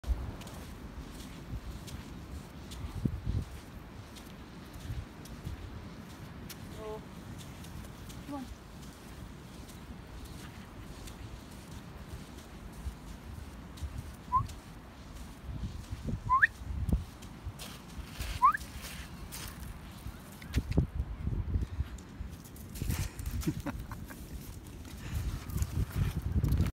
Bedlington Terrier pup Bobby.
Bobby loving his walk on the beach.